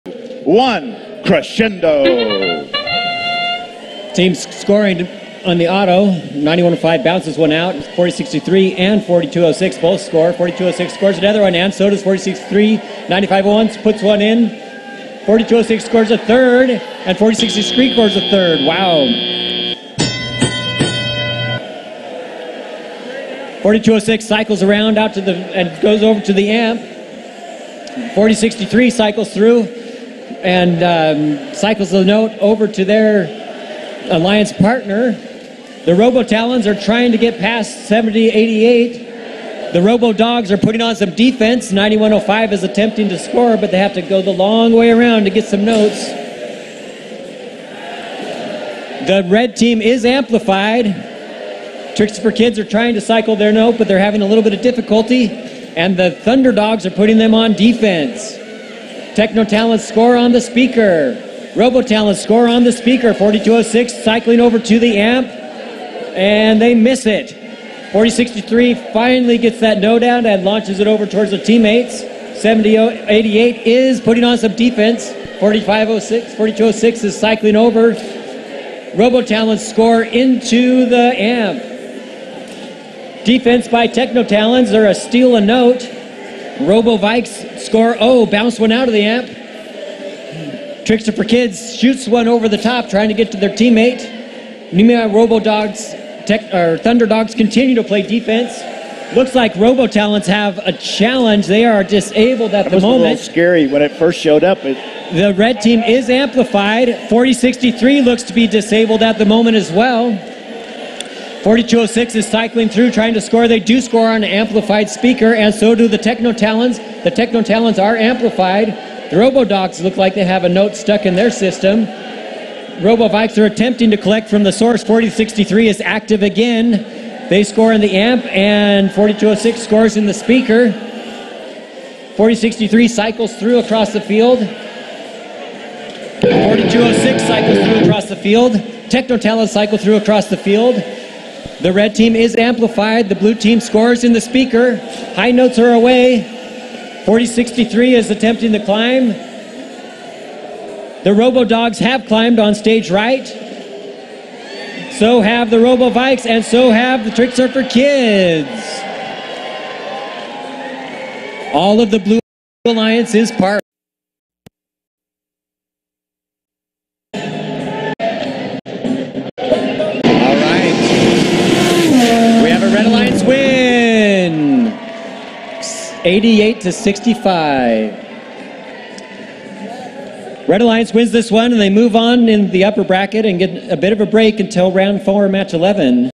Amarillo 2024 M7: TechnoTalons 9105 vs RoboVikes 4206
FiT Texas Amarillo District Event 2024 - Match 7
FIRST Robotics Competition
Date: 3/30/2024

Red Teams: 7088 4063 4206